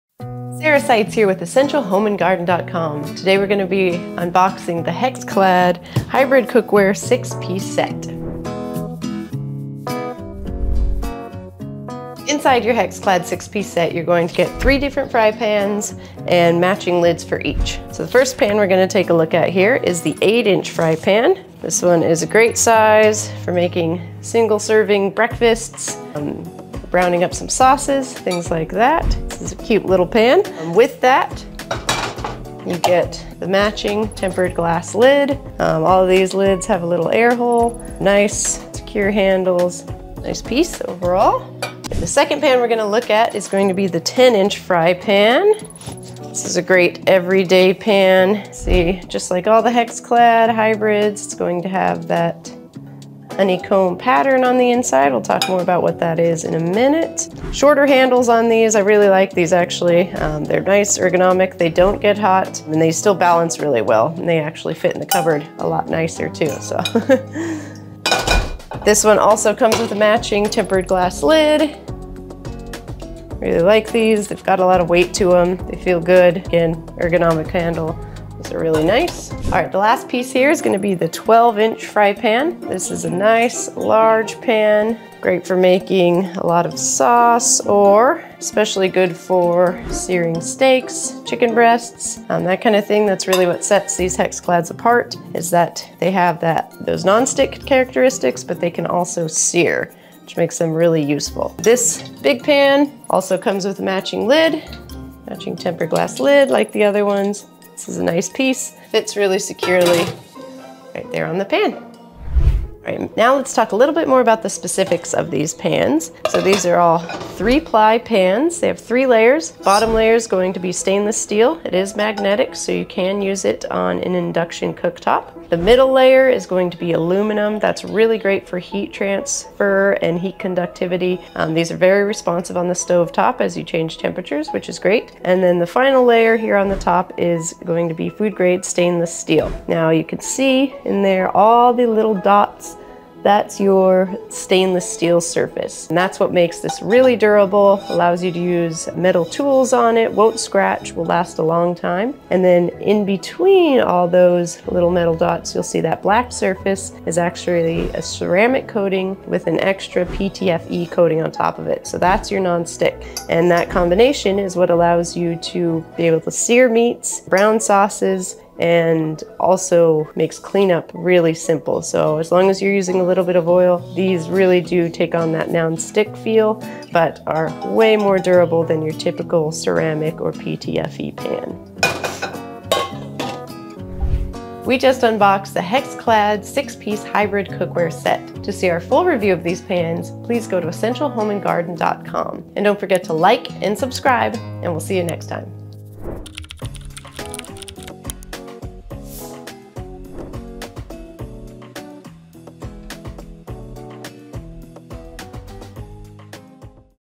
Hexclad Cookware Review - Our Hands On Test and Honest Opinion
In this video we give you a quick review and overview of Hexclad Cookware.

Want to work with us? Want us to look at your products?